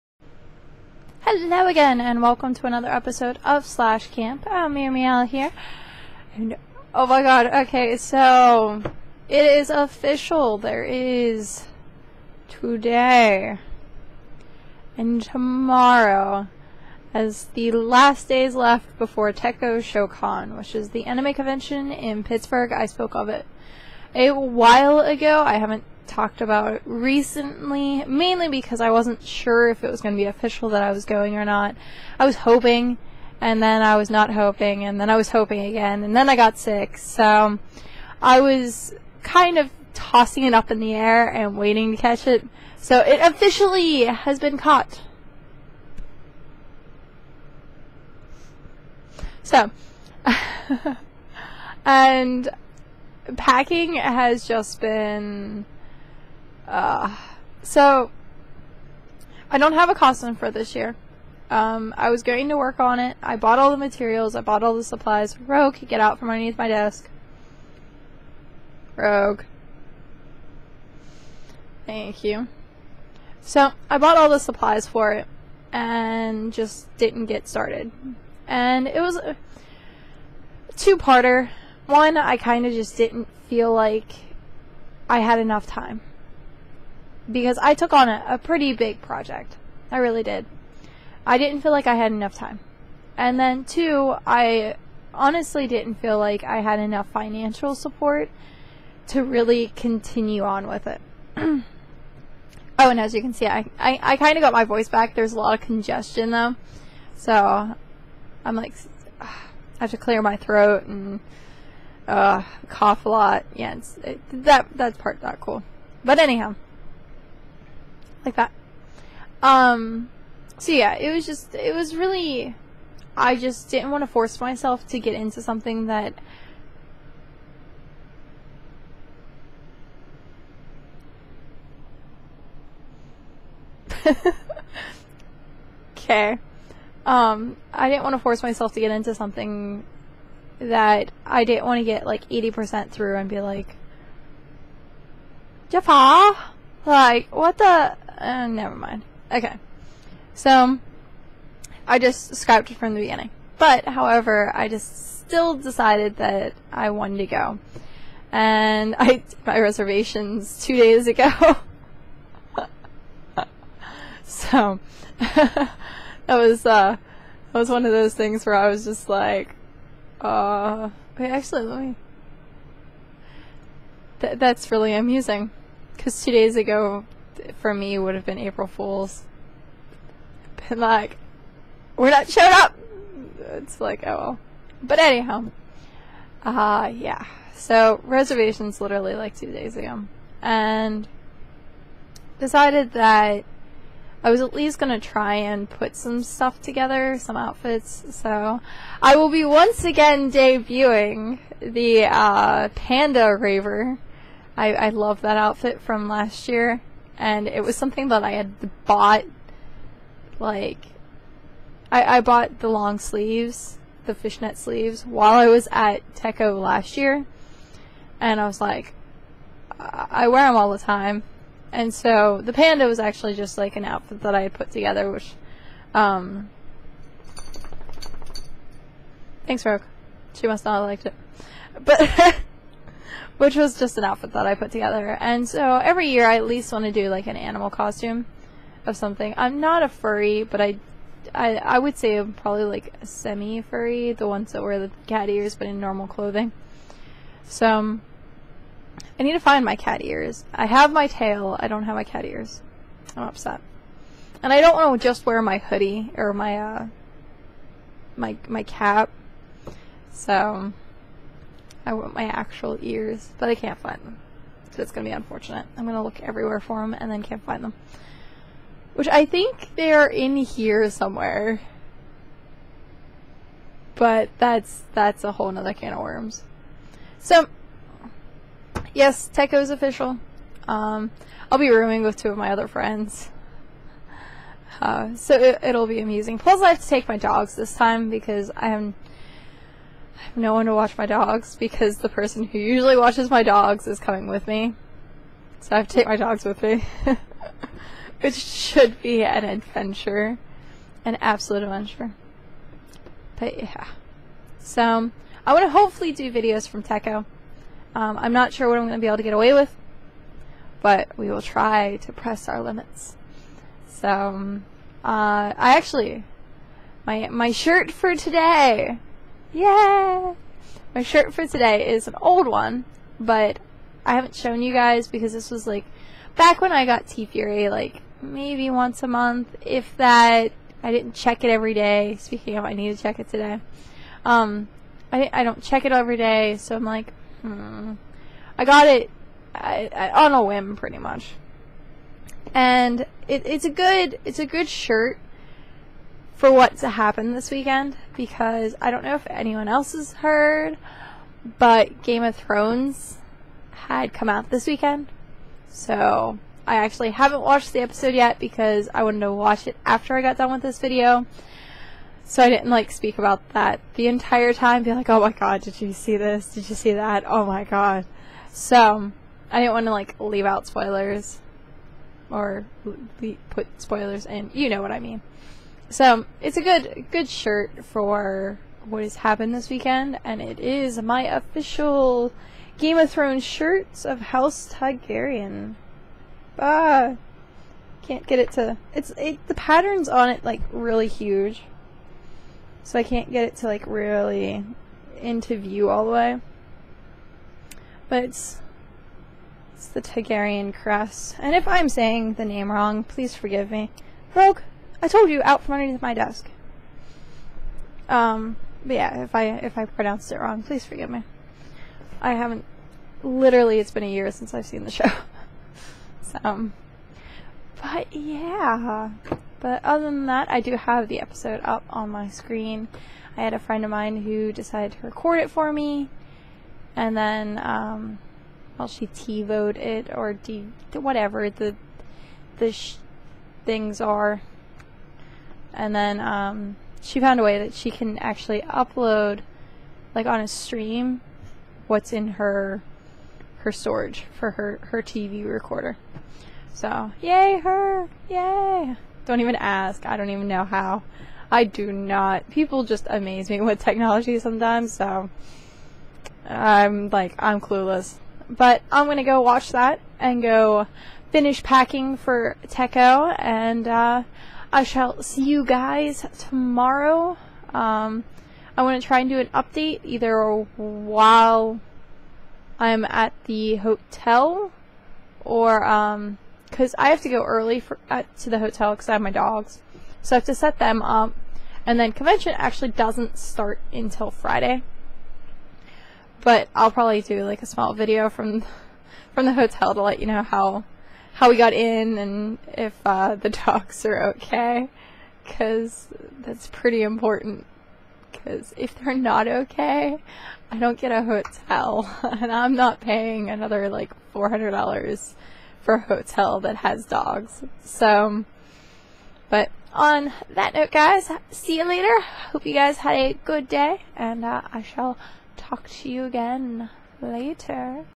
/Camp: Game of Packing
Webisode #31: It's official I'm going to Tekkoshocon this year. Woot woot! So I've started my packing and hopefully everything is okay to head to the hotel room tomorrow. Got to take the dogs with me this year so that should be fun.